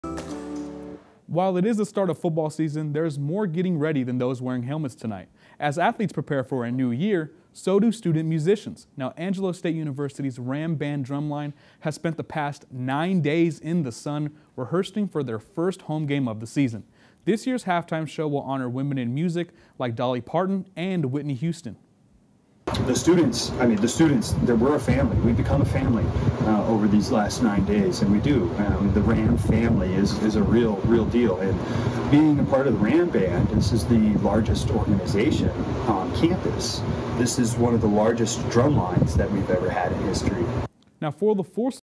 Angelo State University Ram Band Drumline marches into football season
The drumline has been rehearsing for the past nine days in the Texas heat.